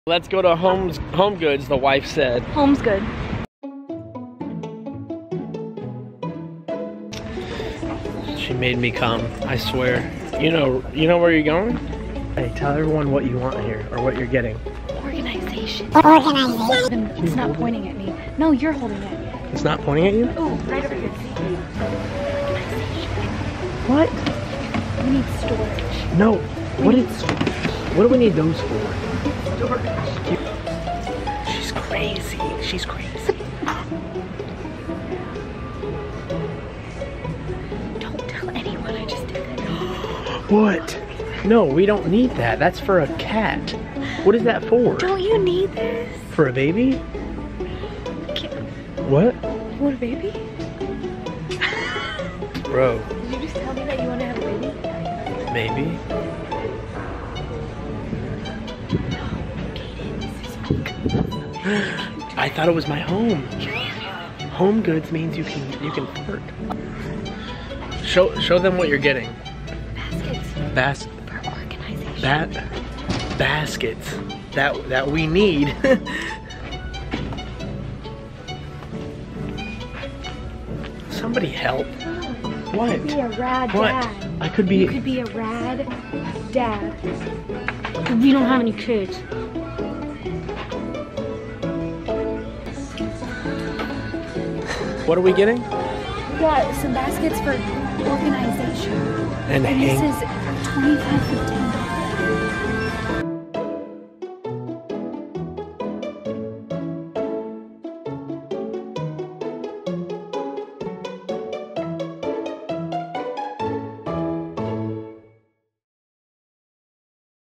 Dayle Takes Kadin To The Goods Of Home
Dayle made me go to homegoods dayle needed to get some Organization things I guess...

📢Also, WE WERE NOT PAID TO MAKE THIS VIDEO!!